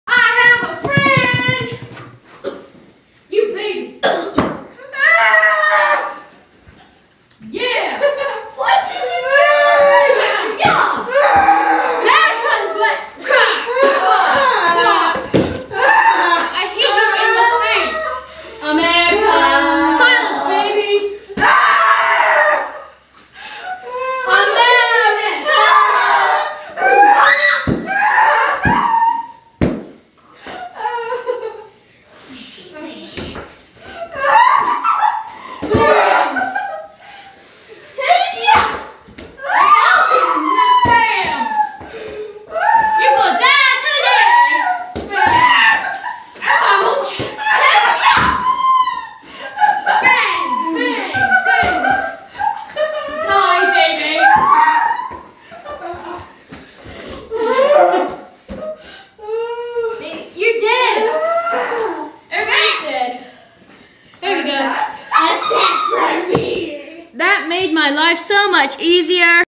Babysitting Skit. xD
i was babysitting and these kids wanted to do a "skit" and i was like hey whatever entertains you xDDD